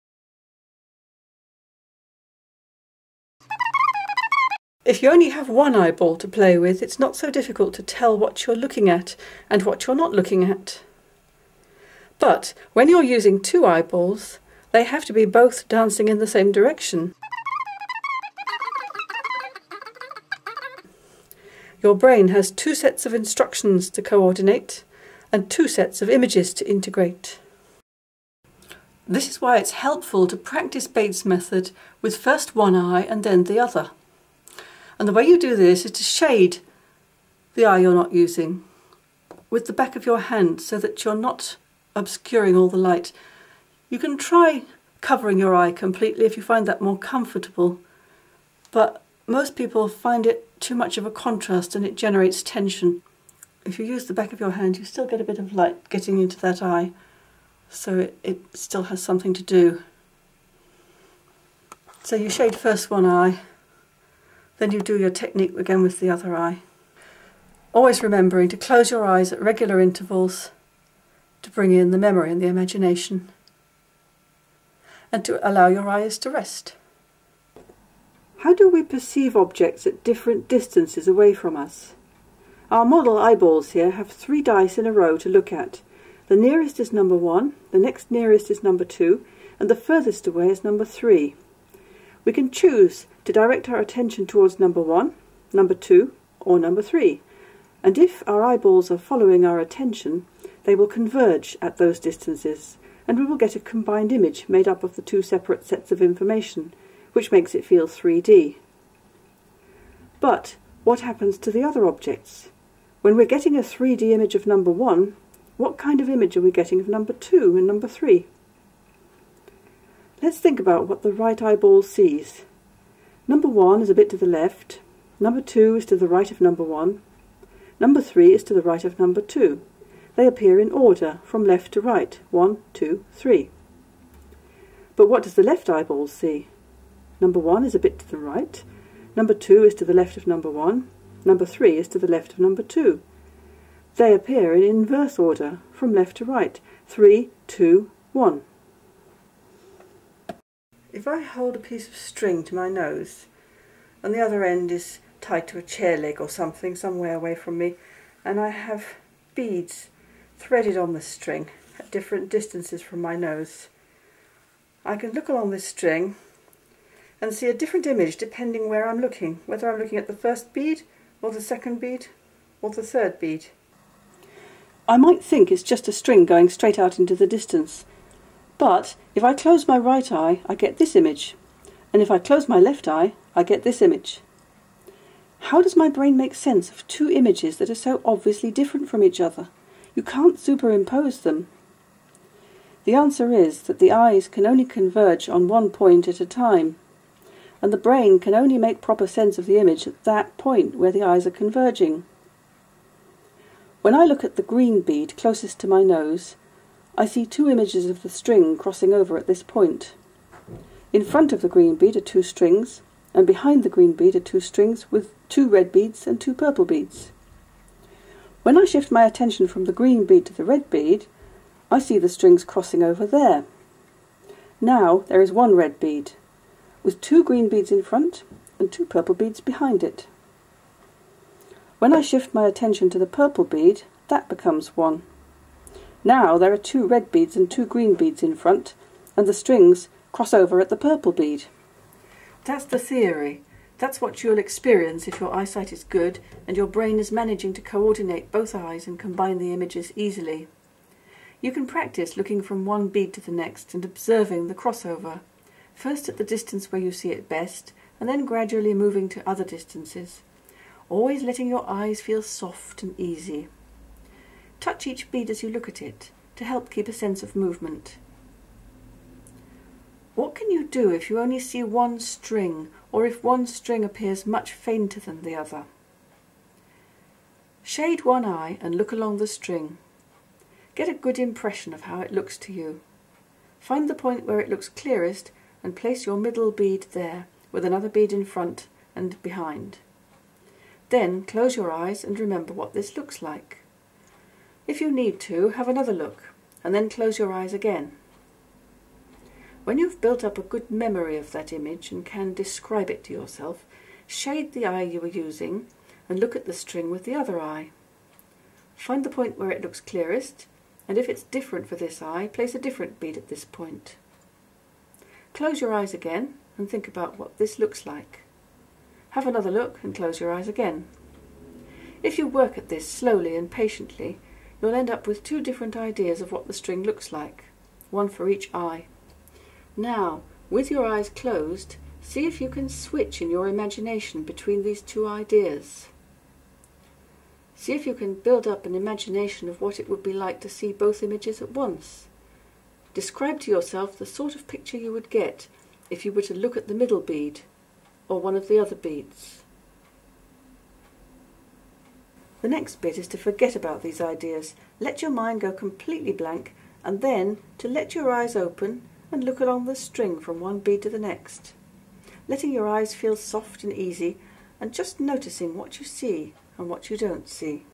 Binocularity and using Bates method with 'Brock string'
When you see with both eyes, your brain has to combine two different images. It has to constantly shift its attention from point to point in order to make sense of what you are seeing.

This exercise uses the 'Brock string' with your memory and imagination to help your brain to build up a stronger image of what is being seen. It can be helpful especially if your one eye doesn't work well with the other, for example if you have a squint or a 'lazy eye'.

You gently shade one eye and practice remembering and imagining. First with one eye, then the other. The string with beads on it helps you to become clear which eye is seeing which image.